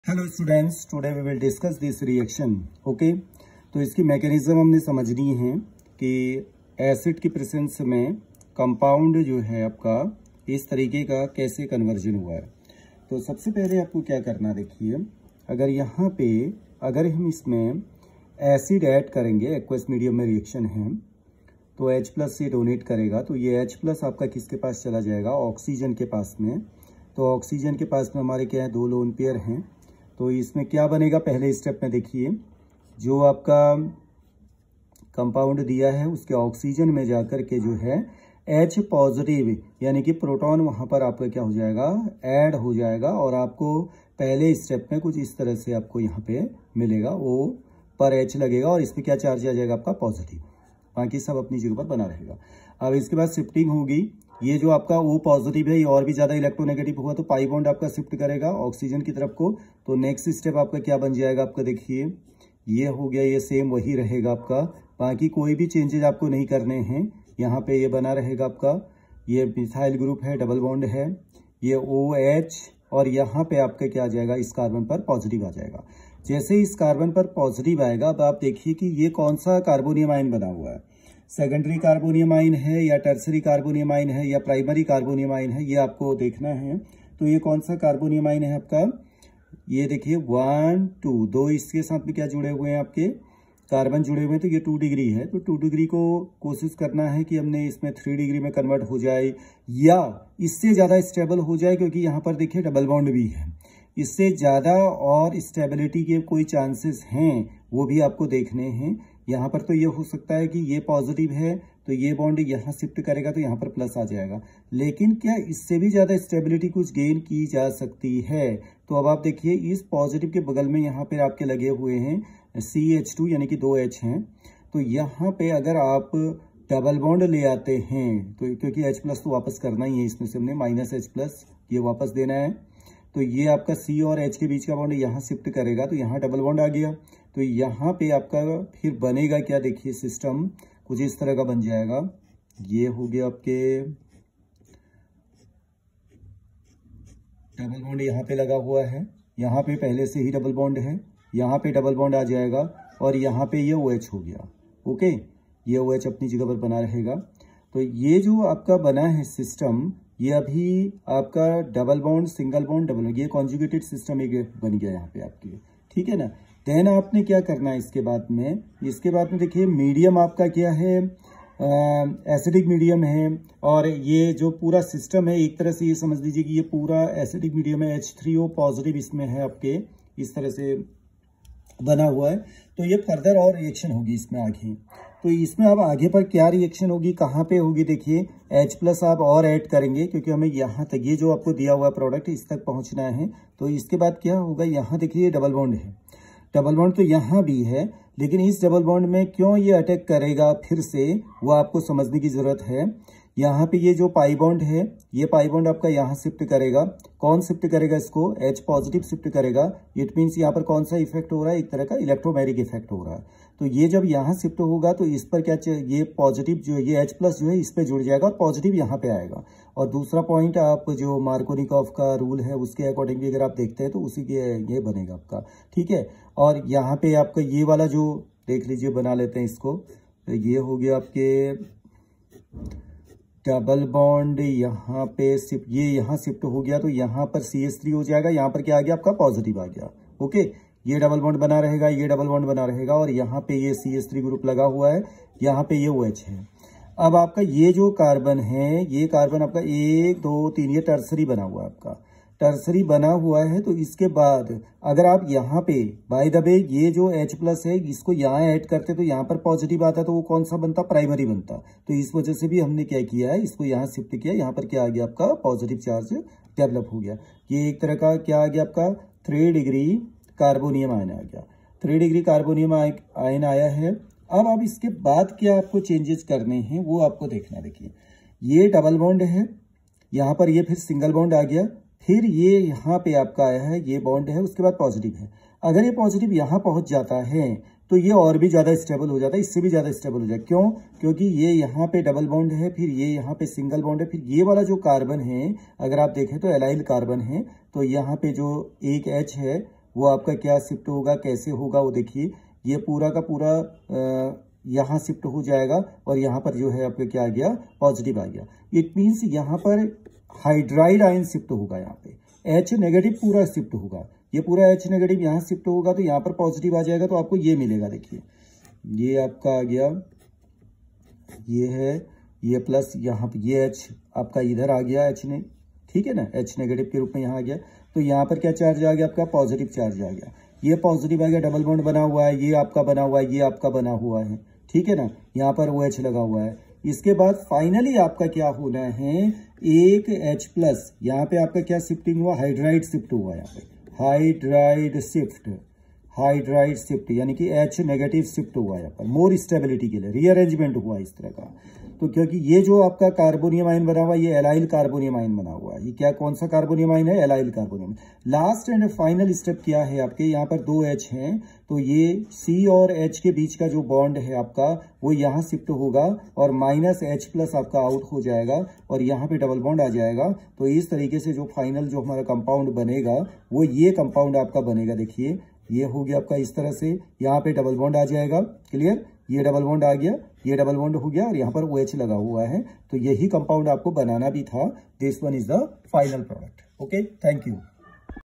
Organic Reaction Mechanism Organic Chemistry BSc JEE CUET NEET Aqueous Medium
Ketones are weak bases that undergo protonation on the carbonyl oxygen in the presence of Brønsted acids. The resulting protonated ketones, called ketonium ions, are strong acids.